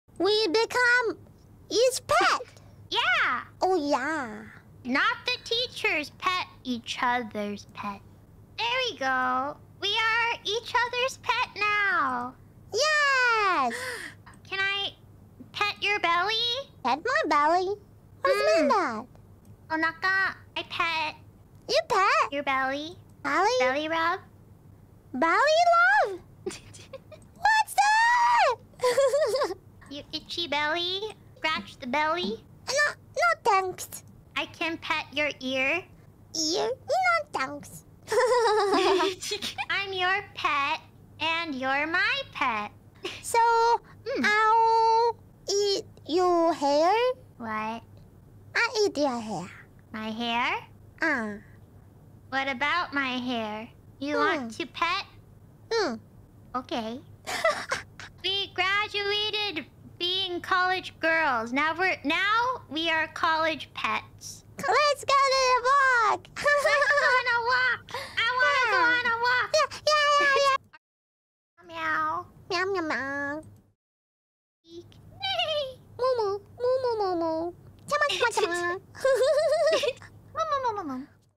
Haachama misuderstands Mumei a bit
01:16 Mumei horse girl leaks out

Here's my twitter if you wanna follow